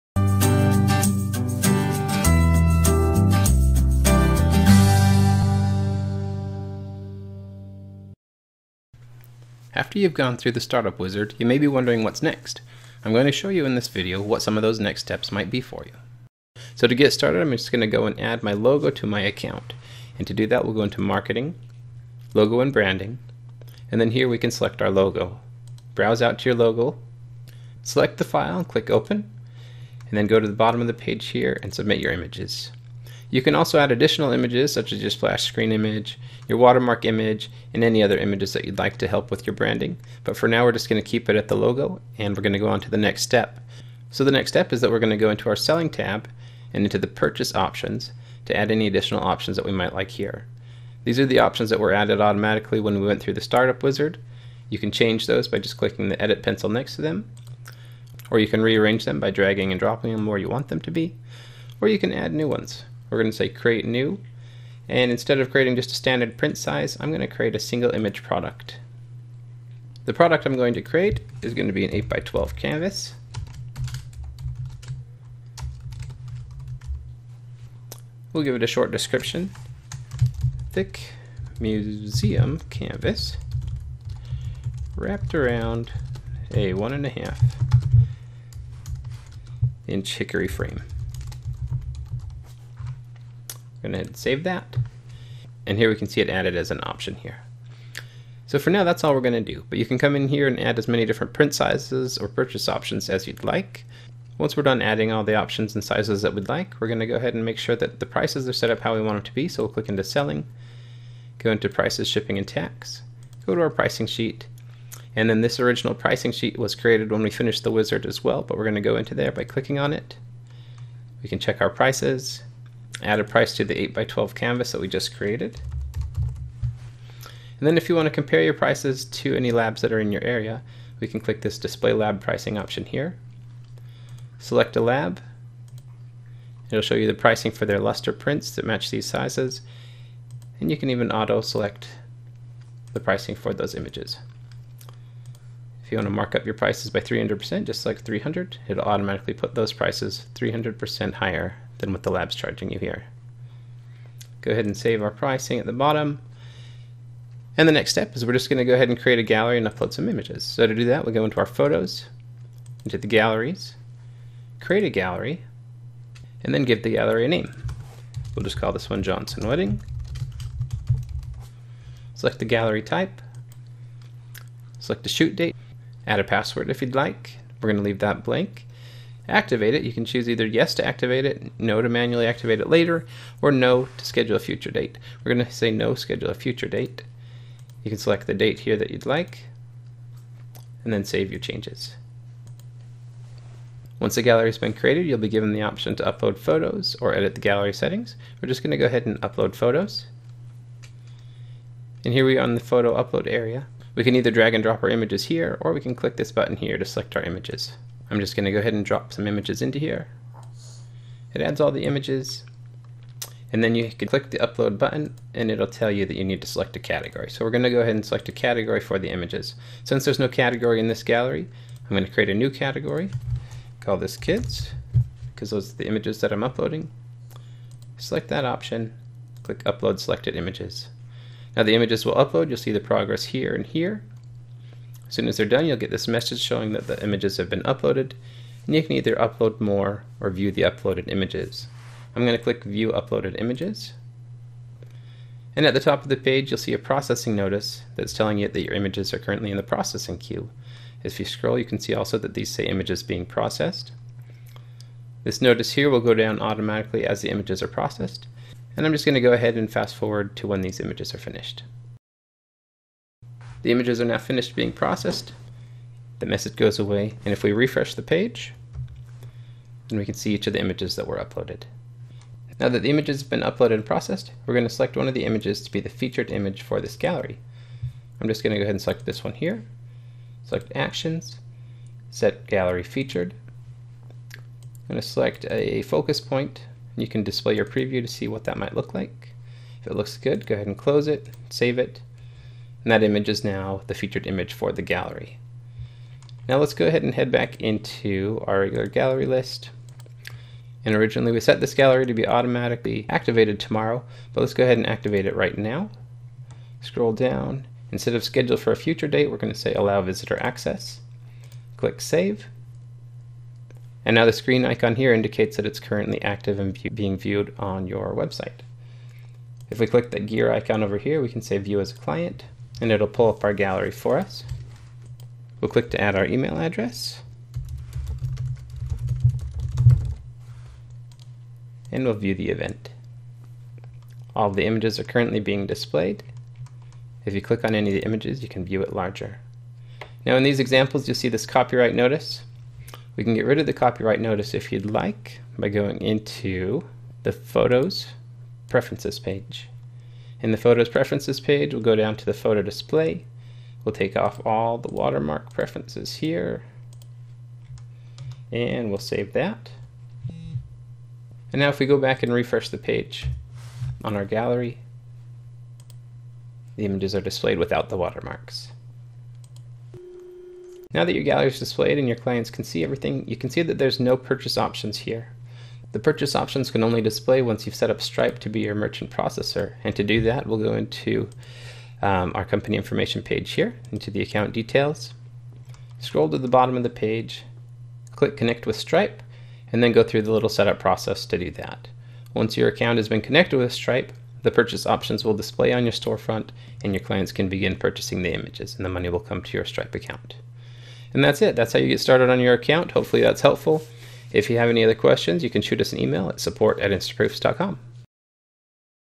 Getting Started. After the Startup Wizard, What's Next?
After you've created your account with Instaproofs and you've finished going through the startup wizard, what's next?  In this video we'll show you exactly what you need to do to get your first gallery up and running after you've finished going through the initial startup wizard.